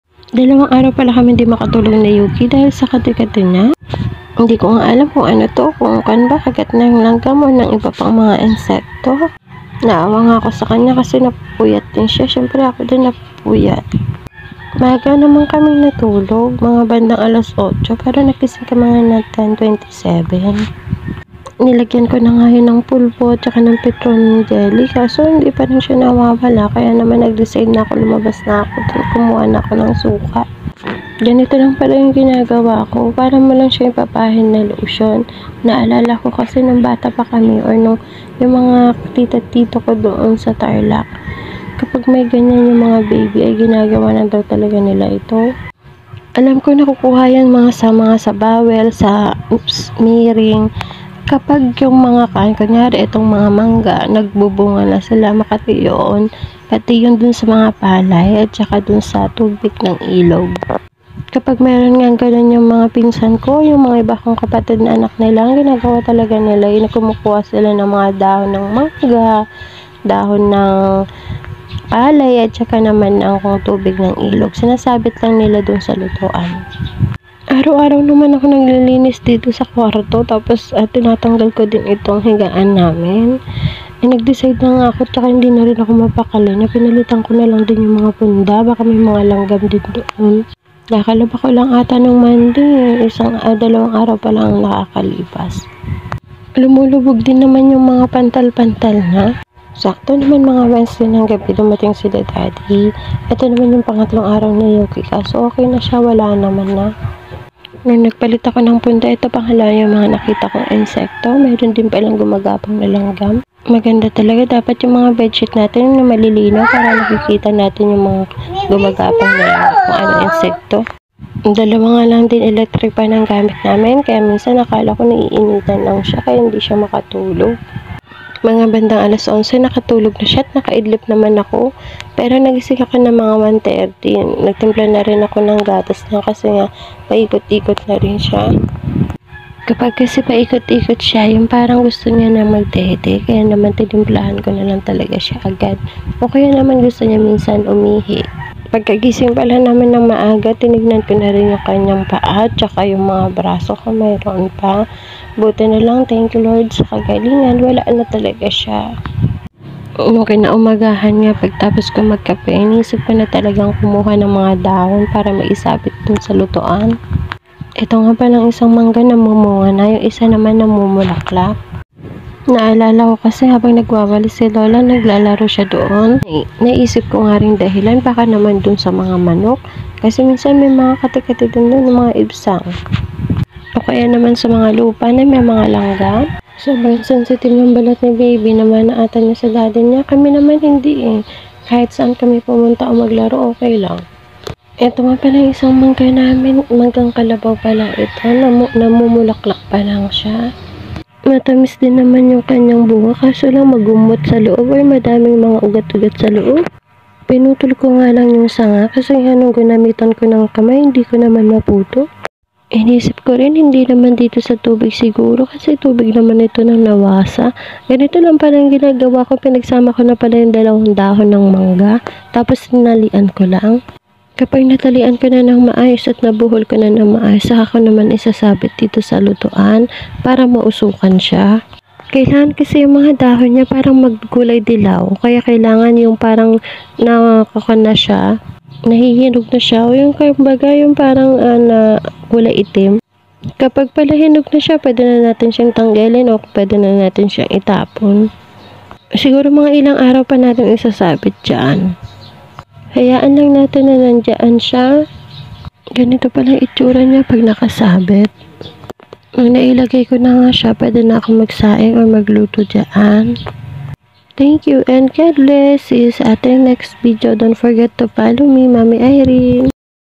Nagkaroon ng mga pantal si Yuukie + Ganito din ba ginagawa ninyo pag nakakaranas sila ng ganito?
My minivlog | Nagkaroon ng mga pantal si Yuukie + Ganito din ba ginagawa ninyo pag nakakaranas sila ng ganito mga momshie?

"Hi guys welcome to my daily minivlog, i hope you like it  our video.Enjoy watching,Thank you and Godbless

Mini vlog. Captions